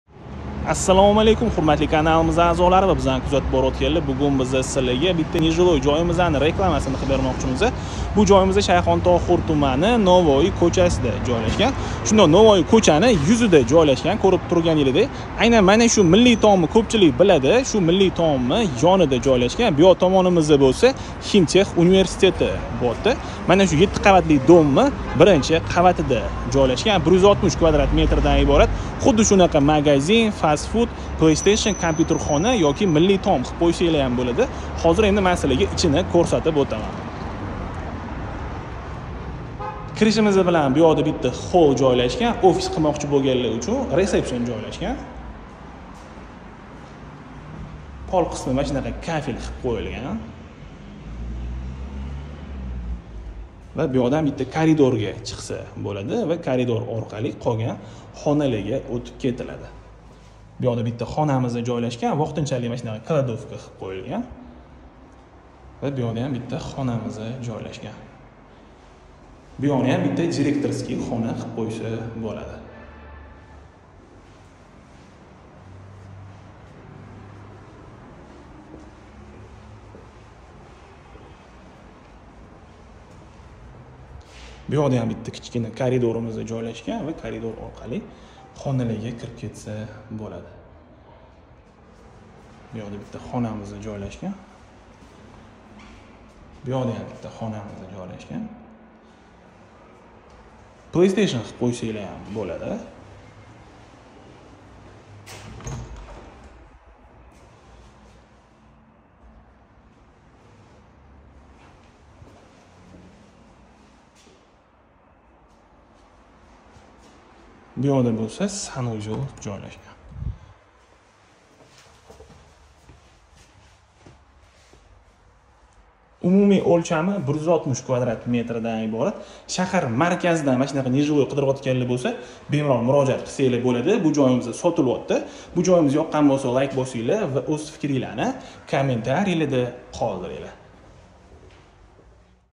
Шахар Марказида 160м² Майдонга Эга Нотурар Жой Сотилади
Шайхонтохур тумани, Навои кўчасида нотурар жой сотилади
160м²
Яхши таъмирланган, катта йўл юзида жойлашган
Пол қисми кафель қилинган, санузел бор
Нотурар жойлар қаторида жойлашган, атрофда барча шароитлар бор
Газ, свет, сув, канализация, интернет бор
Ориентир: Миллий таом, Хим.Тех университети
Нархи: Келишилади